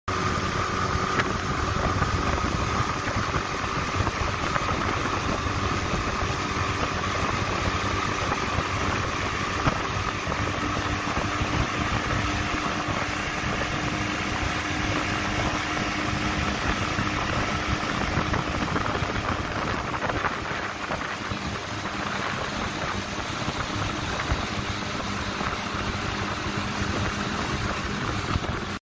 Glen-L Malahini. Boat-builders Gathering 2020/G14
Glen-L Malahini running the TN River, Lake Nickajack during the 2020 Glen-L Boat-builders Gathering.